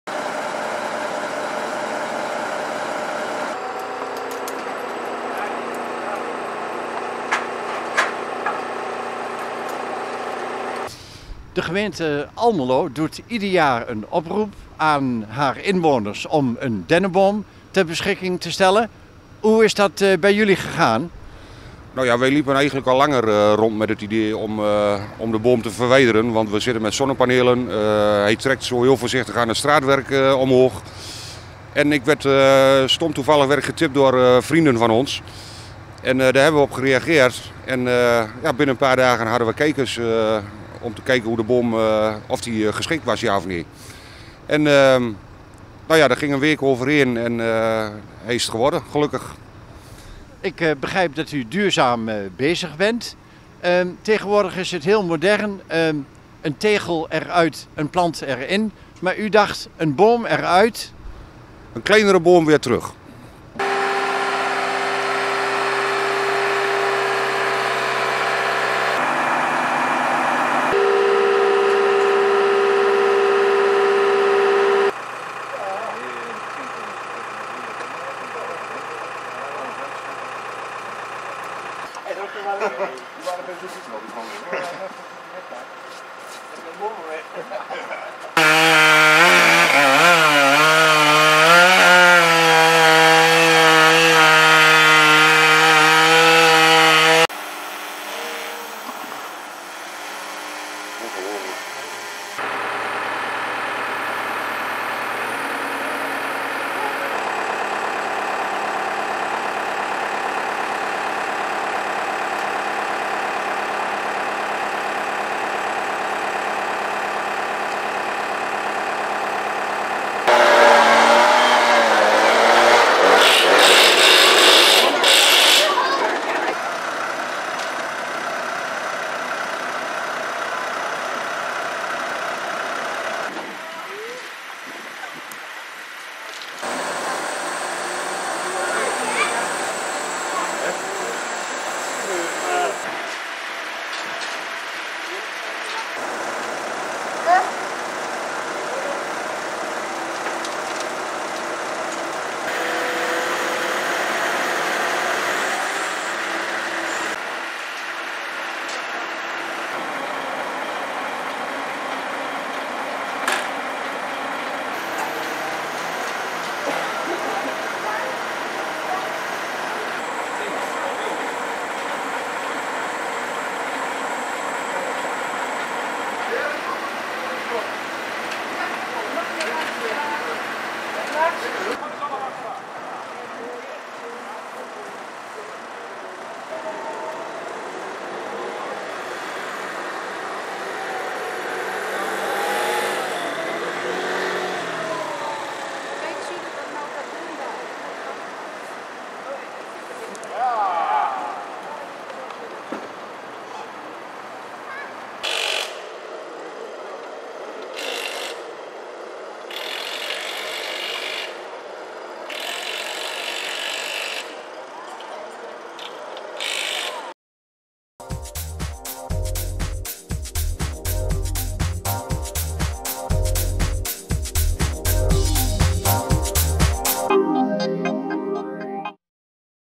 Kerstboom in vijver bij Kreta
Het is ieder jaar een hele operatie die met bijna militaire precisie wordt uitgevoerd: een passende kerstboom die zijn tijdelijke plek krijgt in de vijver bij Kreta.